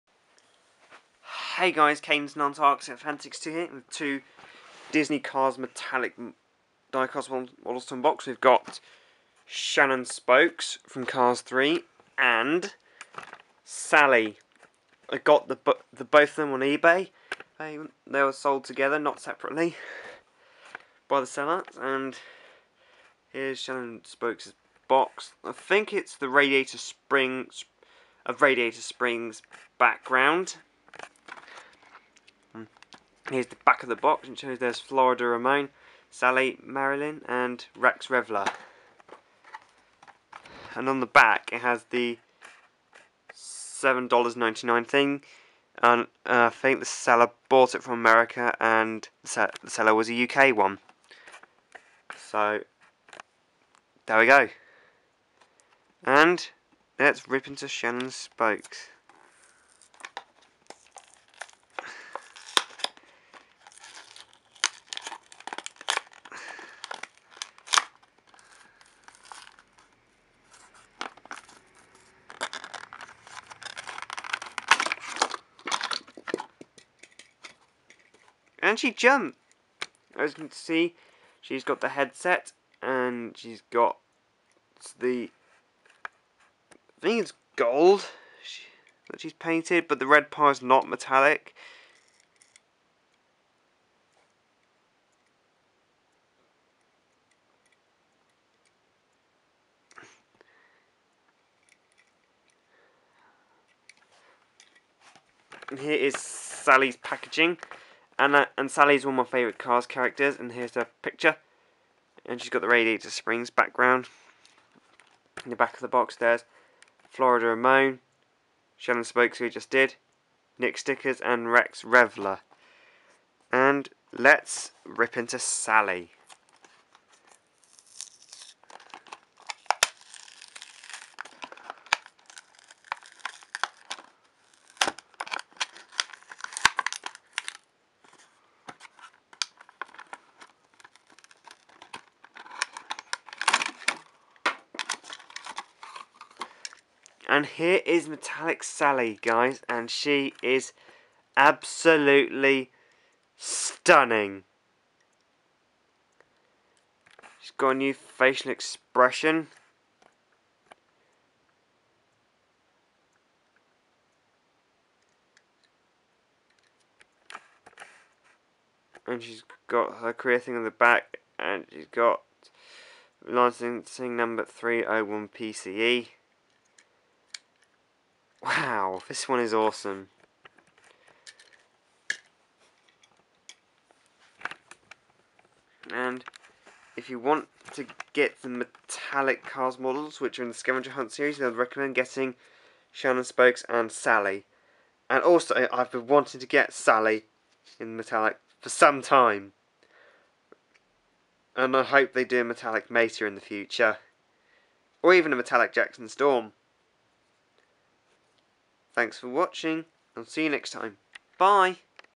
Cars Metallic Sally and Metallic Shannon Spokes unboxing and review
I ordered these two metallic Cars models on eBay on Sunday and they arrived today. They were sold together by a UK seller and I hope you guys enjoy the review.